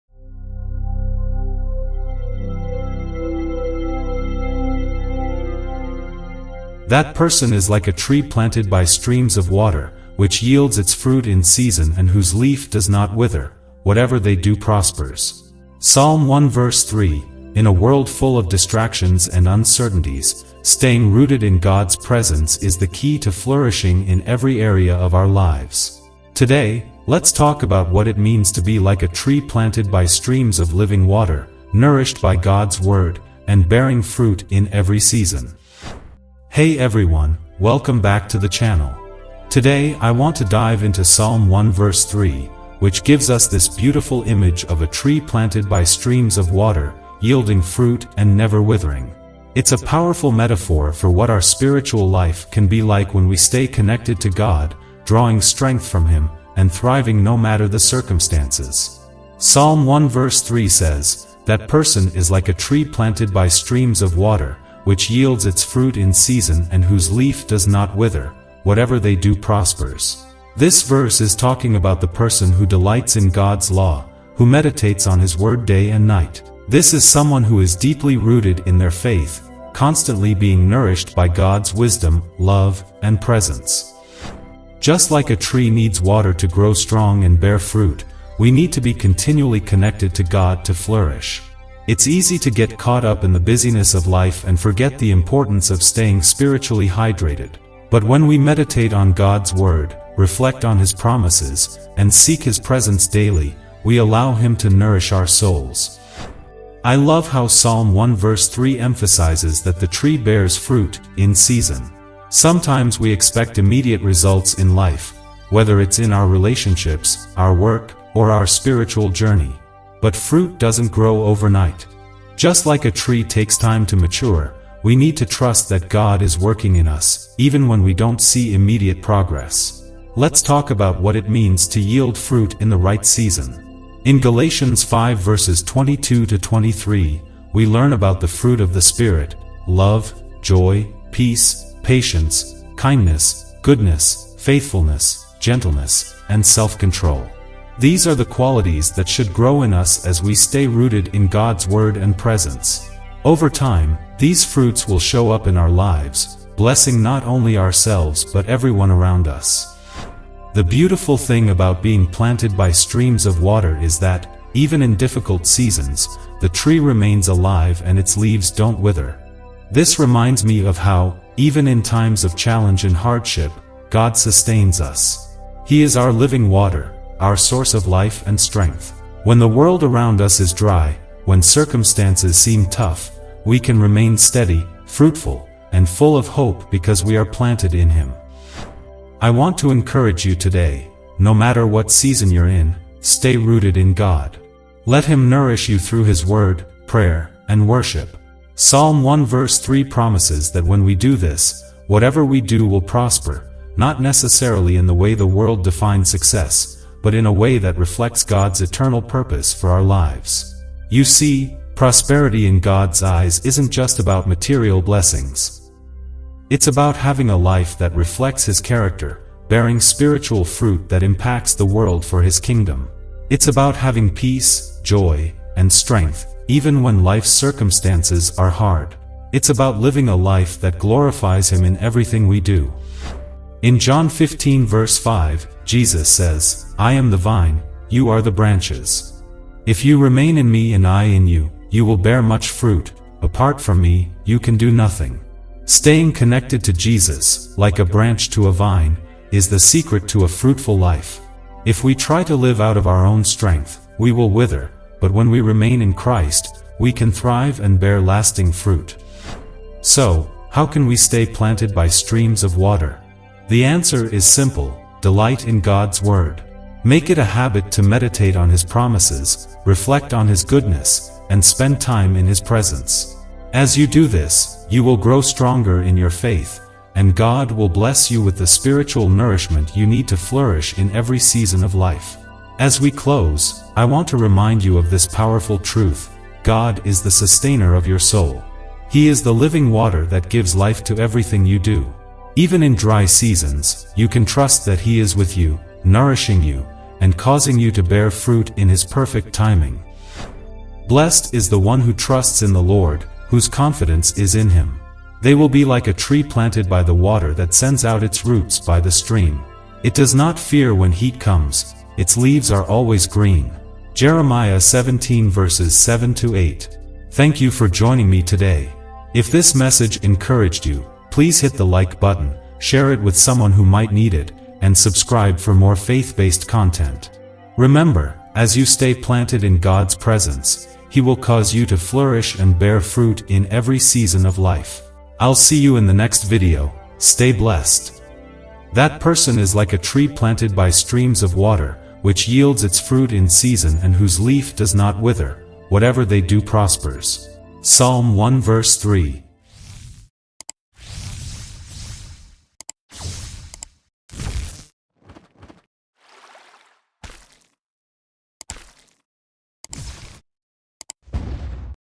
Flourish Like a Tree: The Power of Psalm 1:3 in Your Life
"That person is like a tree planted by streams of water, which yields its fruit in season and whose leaf does not wither—whatever they do prospers." (Psalm 1:3)
In this video, we explore the powerful metaphor of being like a tree planted by streams of water, drawing strength and nourishment from God’s Word. As we meditate on Psalm 1:3, we’ll discover what it means to stay rooted in God, flourish every season, and bear spiritual fruit reflecting His character.
This is a message of hope, encouragement, and a reminder that God is the source of all true prosperity.
📖 Key Bible verses mentioned:
Psalm 1:3
Galatians 5:22-23
John 15:5
Jeremiah 17:7-8

a tree planted by water Psalm 1:3 daily jesus devotional